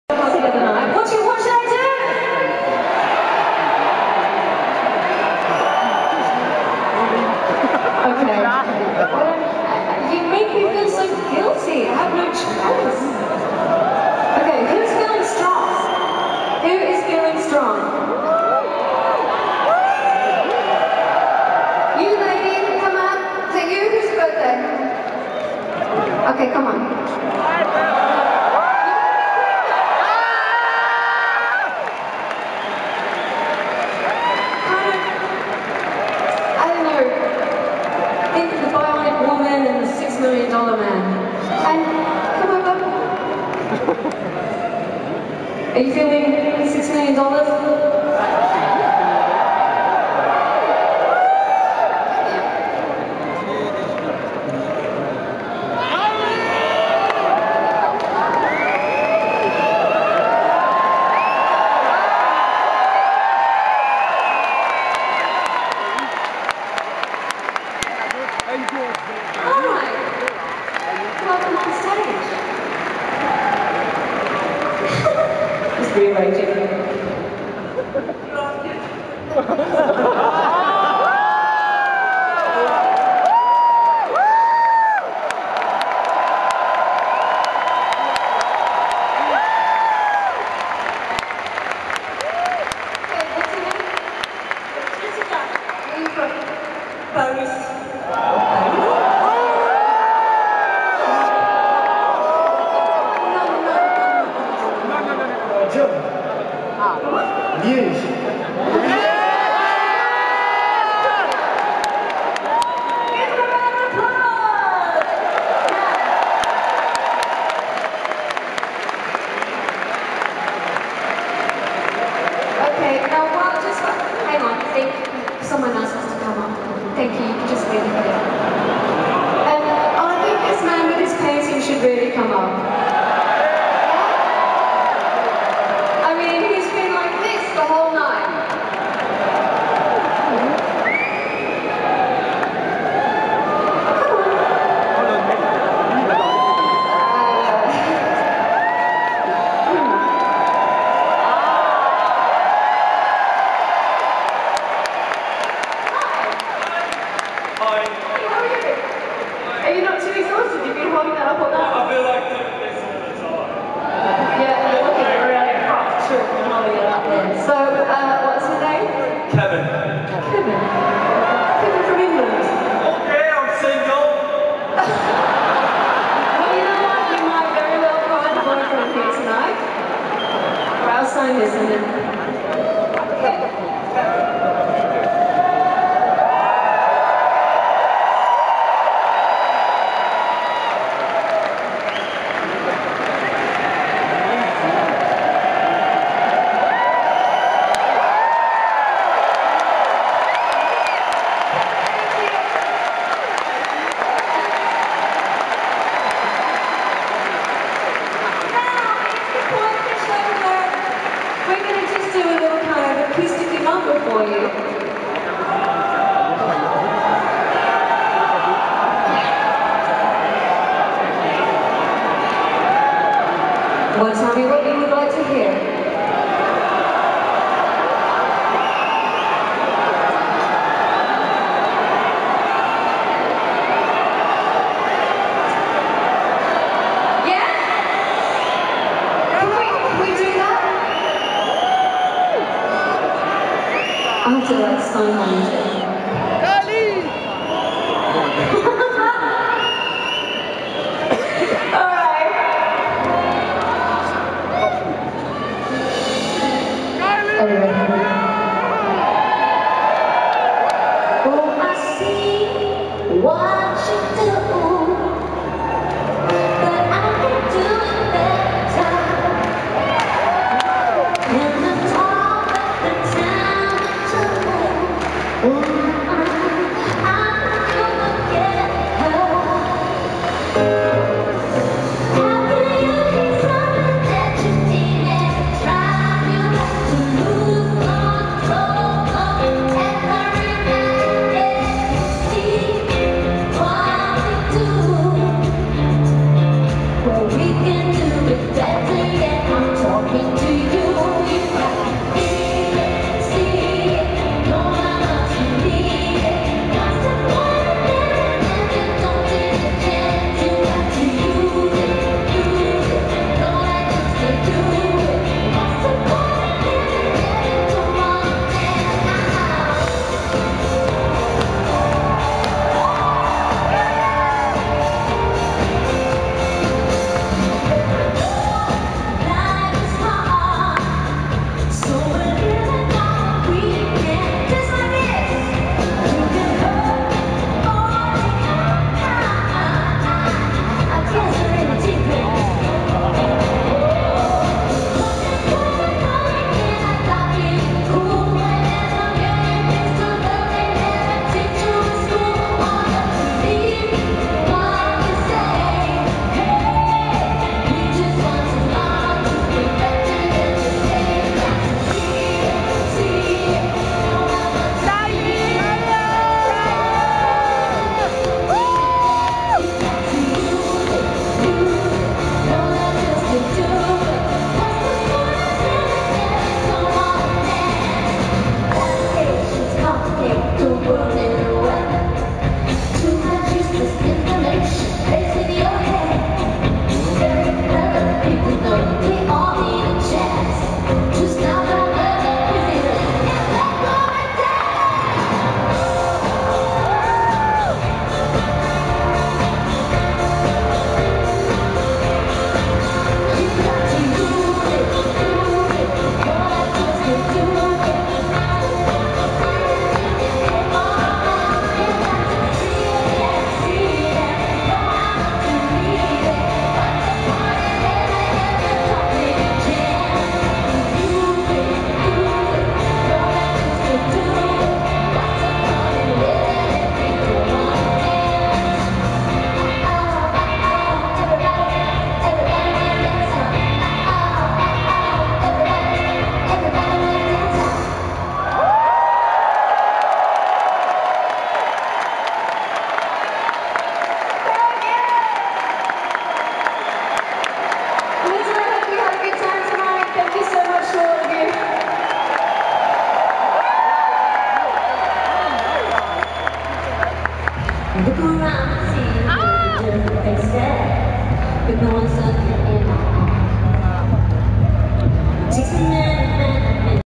Kylie Minogue @ Sportpaleis Antwerp 19-03-2011: Kylie talks to the fans!
Kylie talks to the fans, and asks 3 of them on stage!